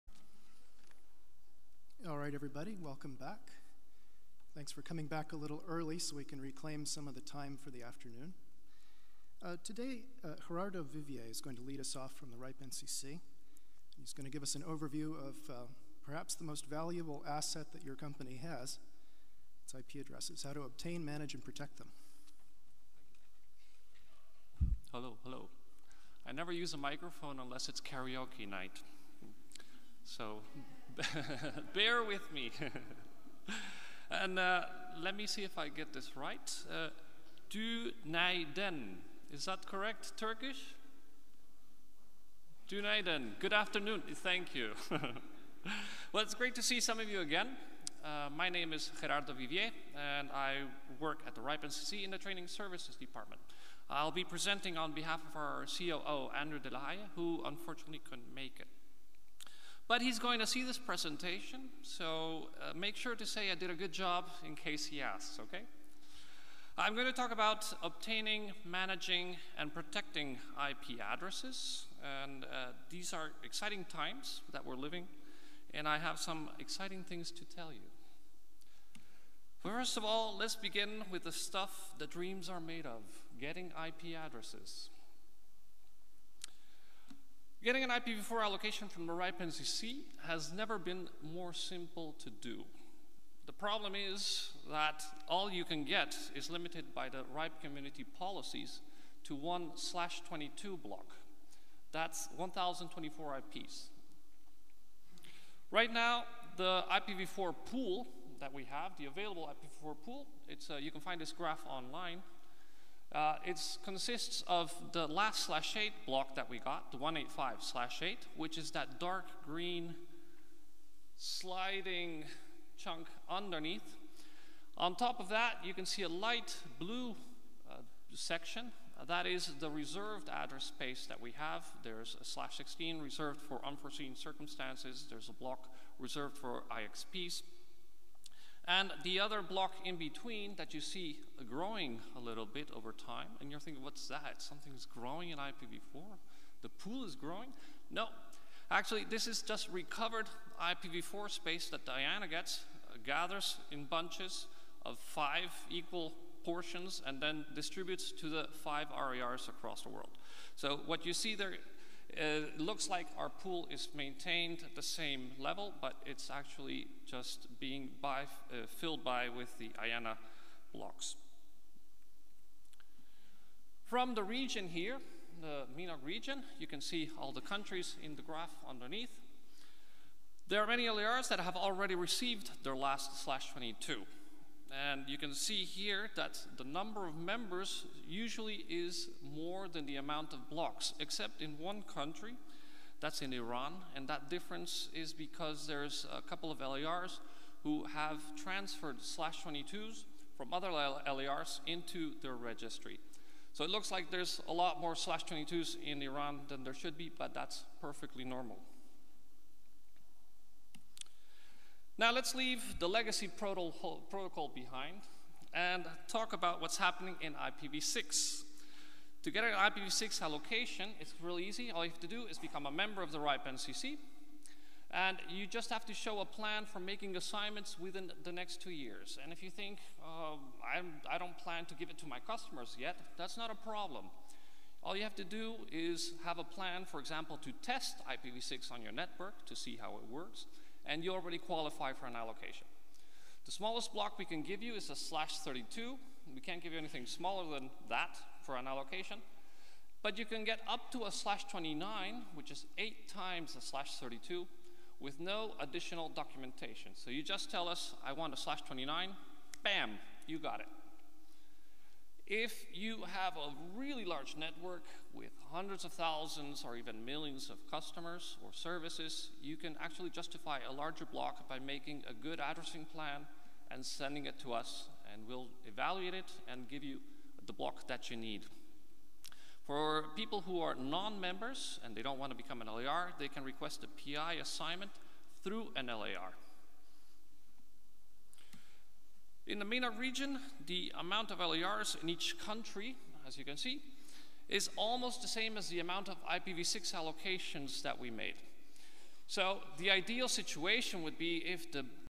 MENOG 16: Obtaining, Managing and Protecting IP Addresses (part 1) - Gerardo Viviers (RIPE NCC)
Obtaining, Managing and Protecting IP Addresses - Gerardo Viviers (RIPE NCC)

MENOG 16 took place in Istanbul from 23-24 March 2016.